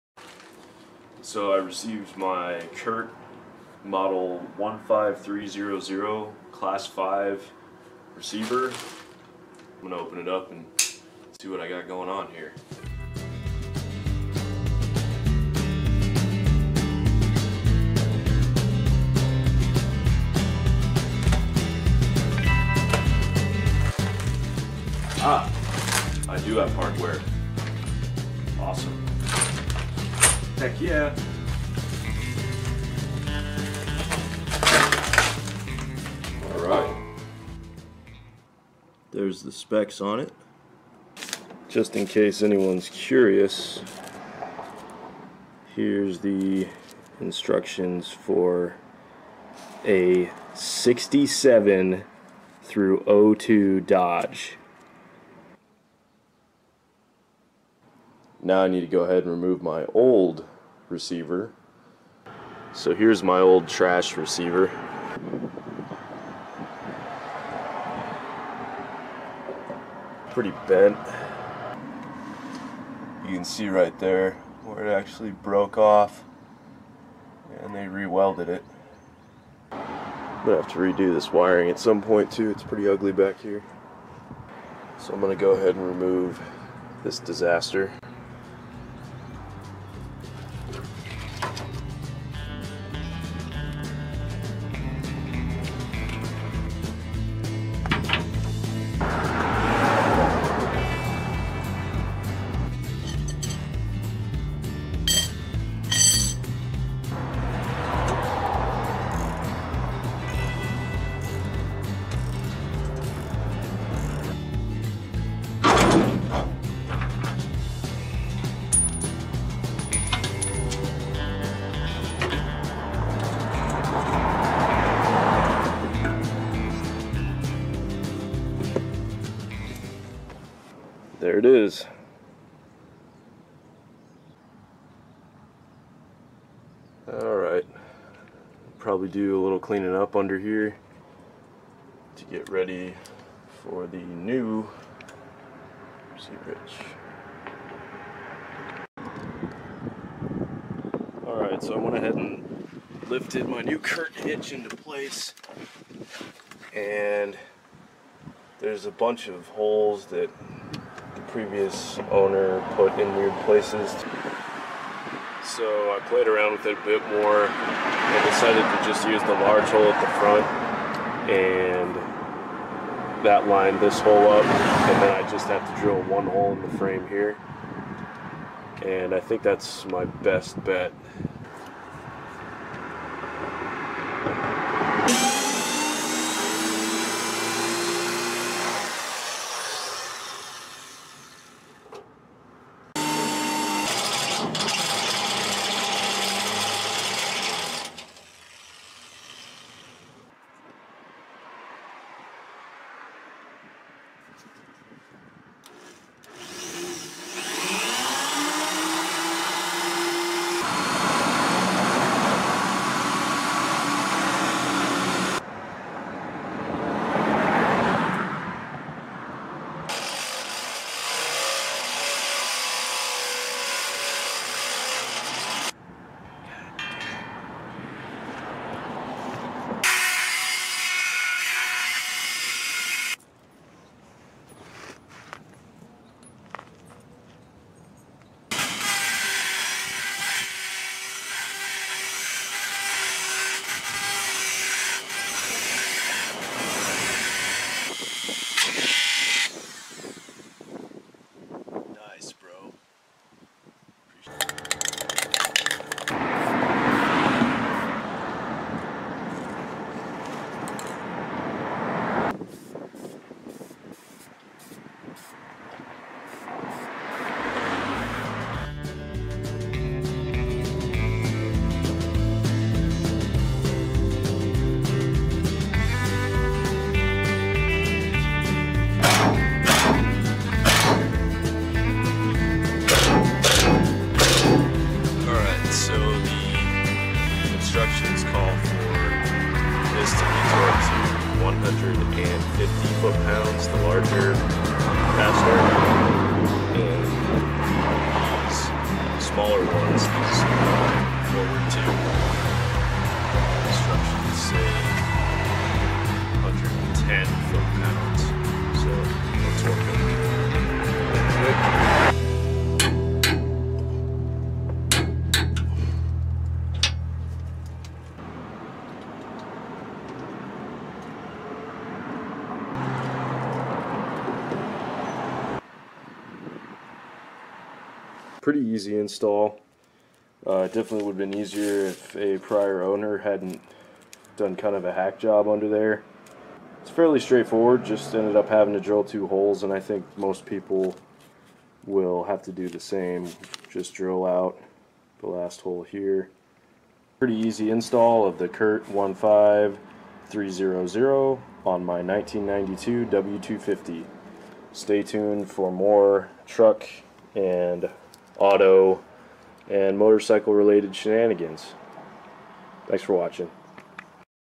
Curt 15300 Receiver Hitch 1st Gen Dodge Cummins Install
Just a quick install vid of a new receiver for my 1992 W250 Cummins